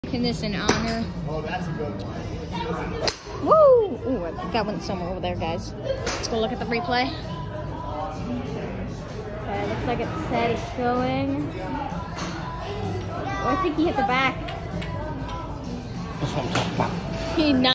Mike Trout we are coming for you!! Top golf Back of net!!
Hit the back of the net at top golf!!